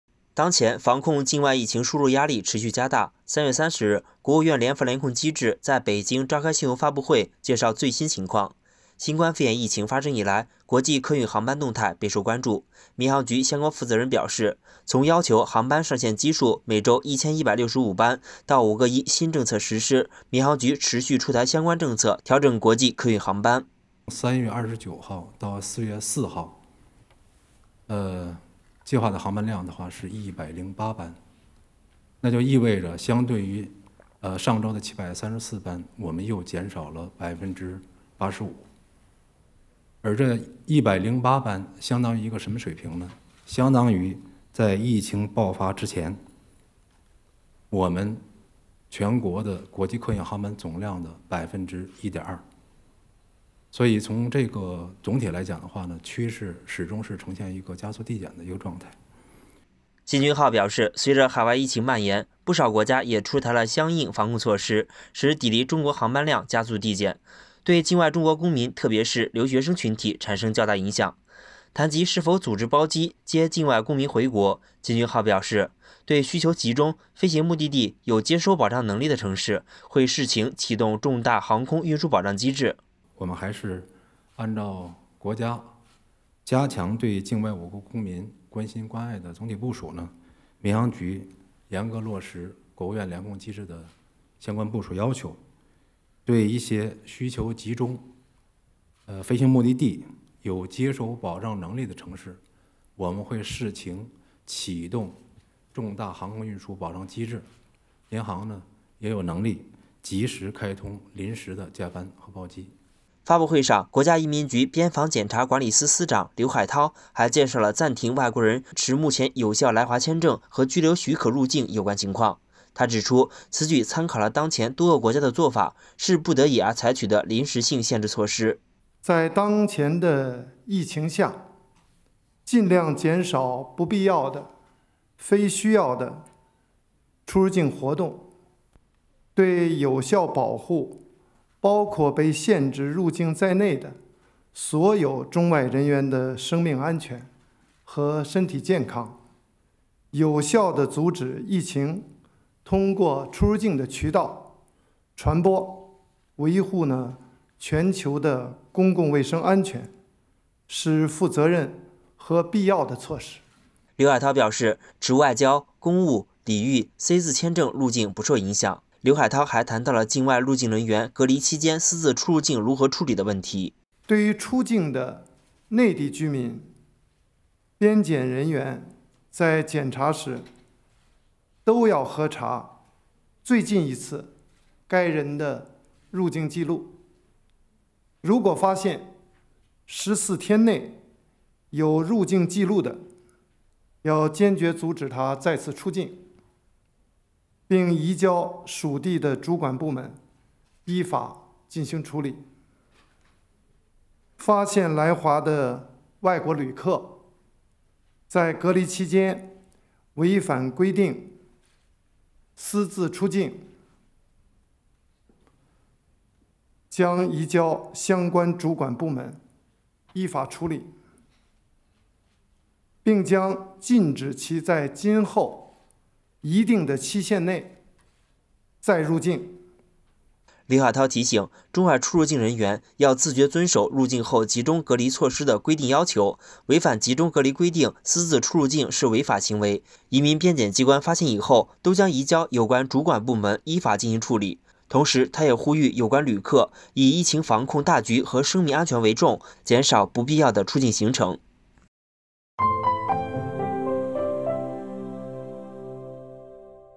是否包机接回境外公民？隔离期间私自出入境如何处理？相关部门回应
当前，防控境外疫情输入压力持续加大，3月30日，国务院联防联控机制在北京召开新闻发布会，介绍最新情况。国际客运航班动态备受关注。民航局相关负责人表示，从要求航班上限基数每周1165班，到“五个一”新政策实施，民航局持续出台相关政策调整国际客运航班。